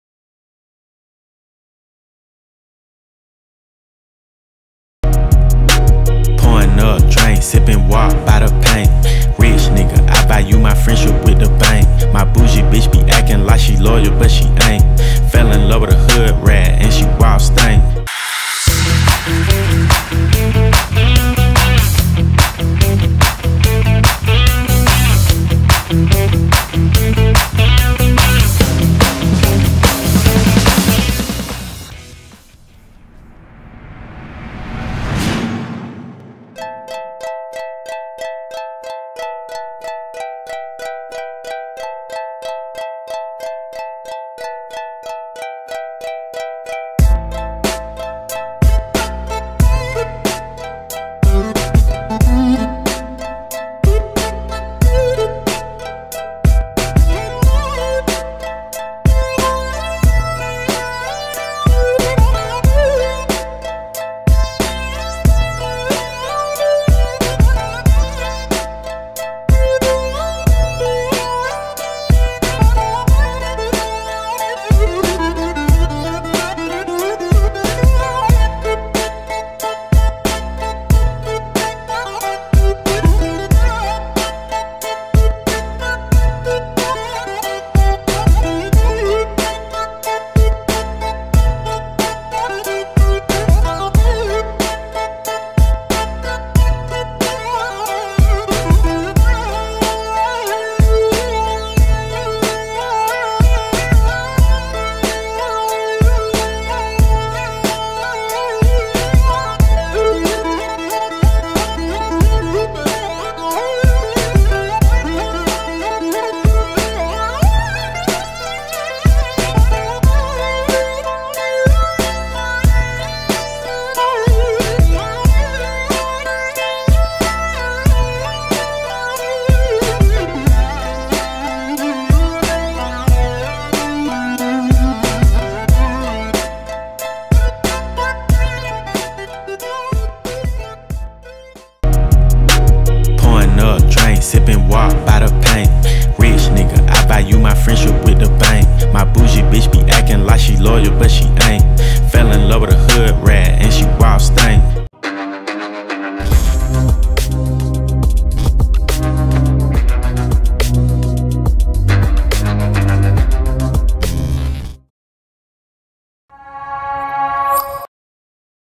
How To Create A Lyric Video (Tutorial)
How to create a lyric video in wondershare filmora. make sure you watch the video until the end . you can support a creator through subscribing , like the video or even sharing the video link provided down below